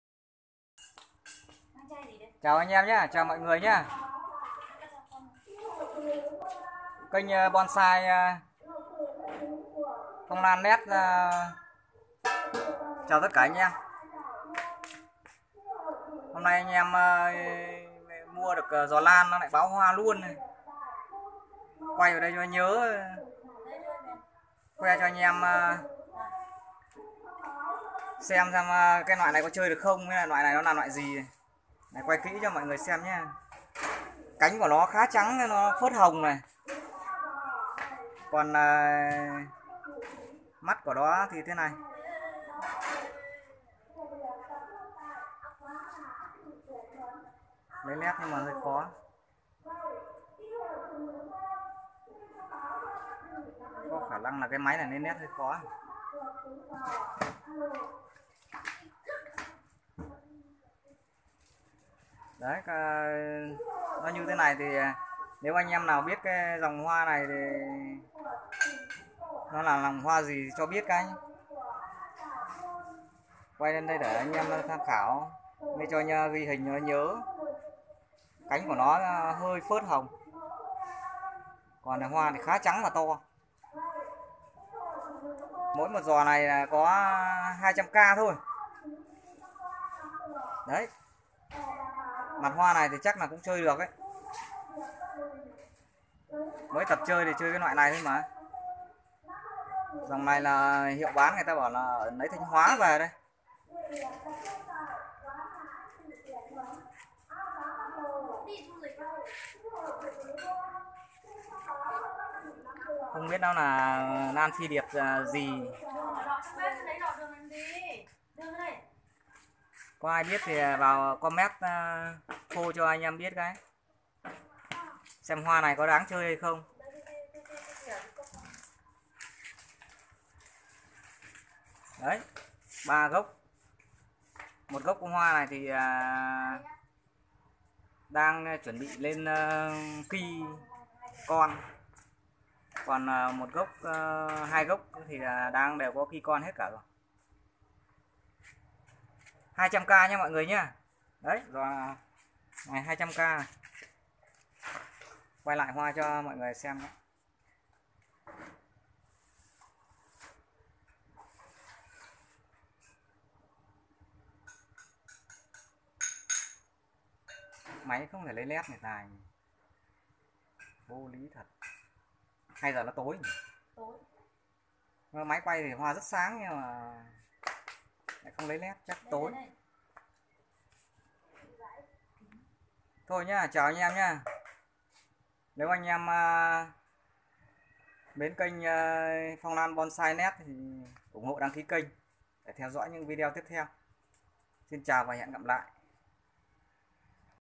Hoa bói L Phi điệp mua hàng chợ 200k có đáng chơi?
Giò lan phi điệp mua 200k ở cửa hàng vật tư Lan  có hoa anh chị em cô Bác ai xem mặt hoa nếu biết xem mặt hoa này là loại gì có đáng chơi không tư vấn giúp mình.